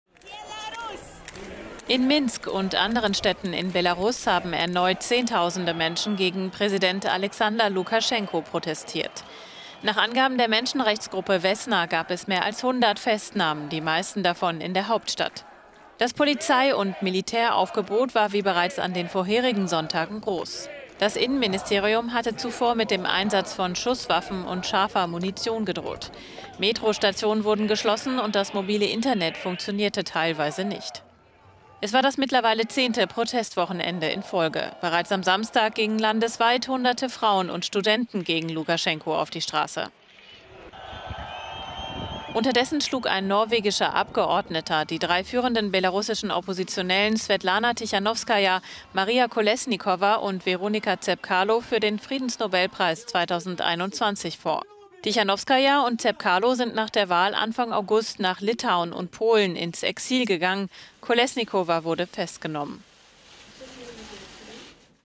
10. Protestwoche: Zehntausende demonstrieren gegen Lukaschenko
In Minsk und anderen Städten in Belarus haben erneut Zehntausende Menschen gegen Präsident Alexander Lukaschenko protestiert.